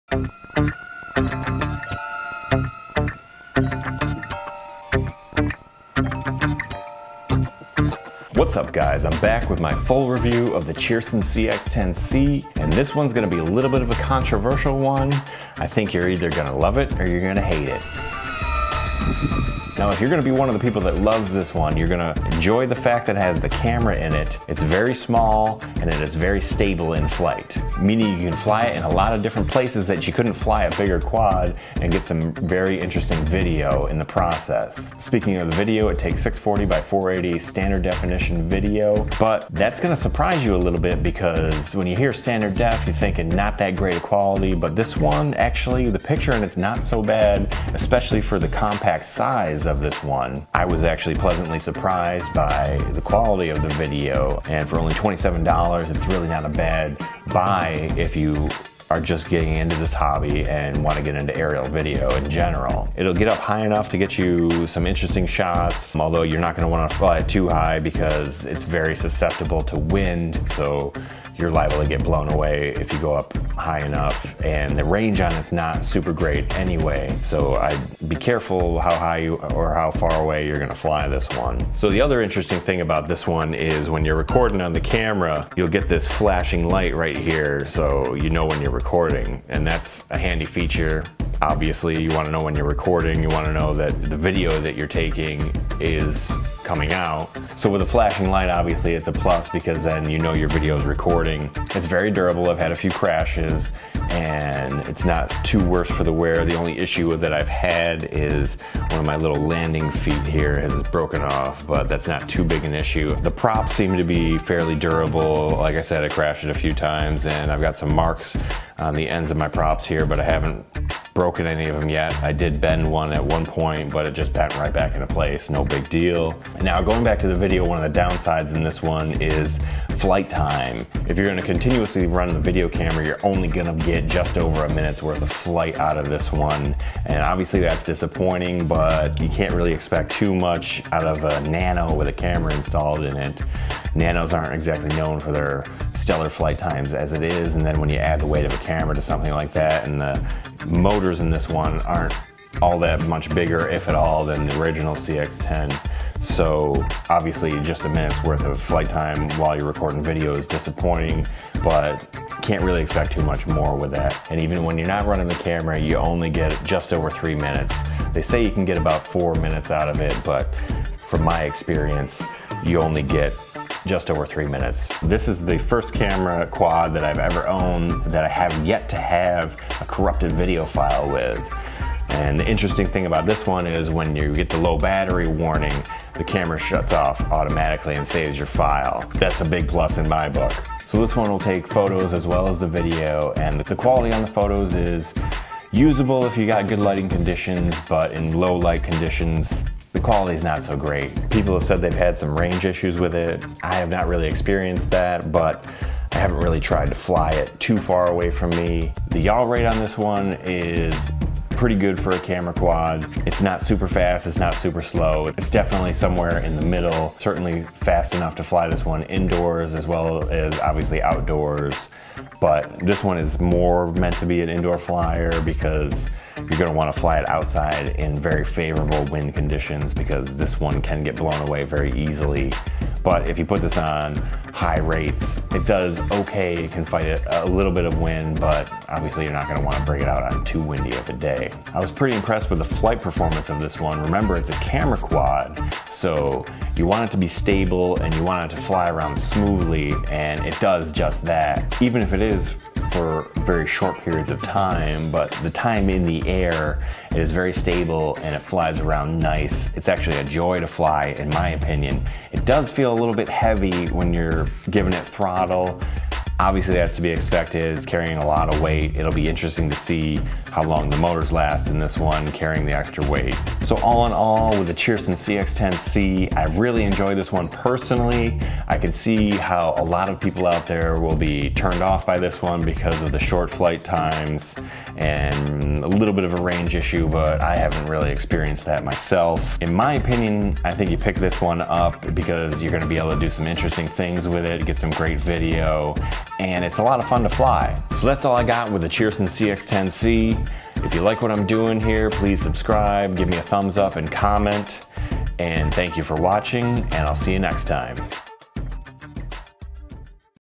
Cheerson CX-10C: World's Smallest Camera Quad Review
My full review of the Cheerson CX-10C, the world's smallest camera quad.